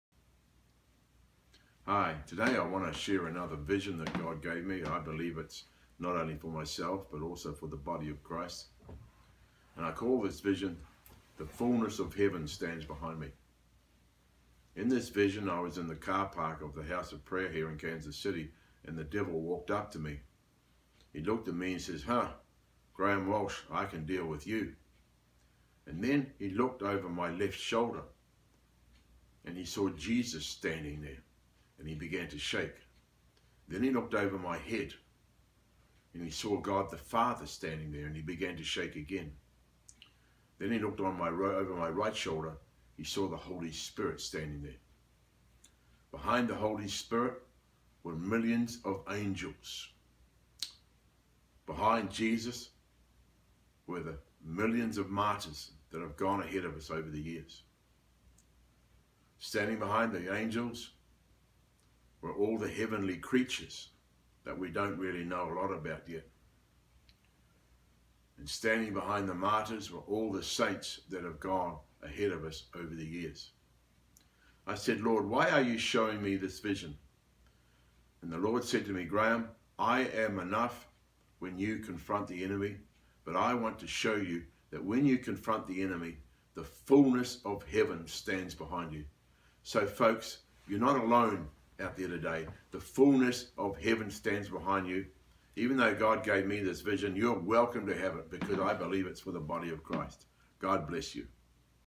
The Fullness Of Heaven Stands Behind Us
Are you aware the hosts of heaven are standing with you. Graeme was given a vision from the Lord to encourage us all that we don't stand alone; all of heaven is with us.